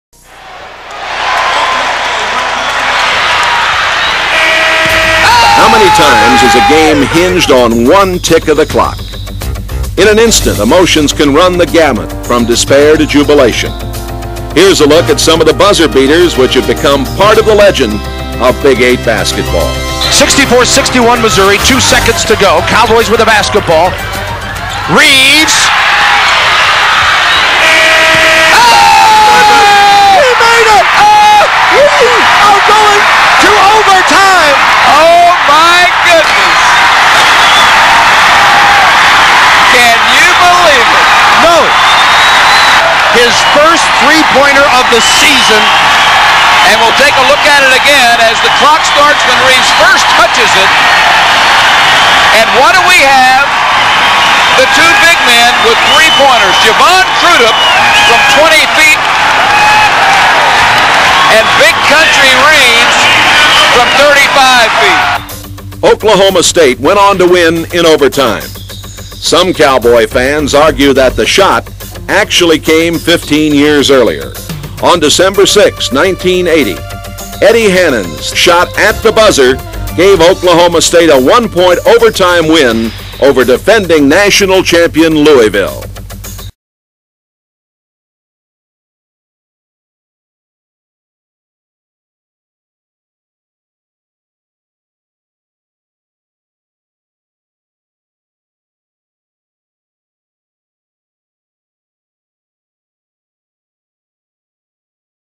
OSU Buzzer Beaters
A look at two of the moments that live in Oklahoma State basketball lore...Bryant Reeves' and Eddie Hannon's miracle half-court shots.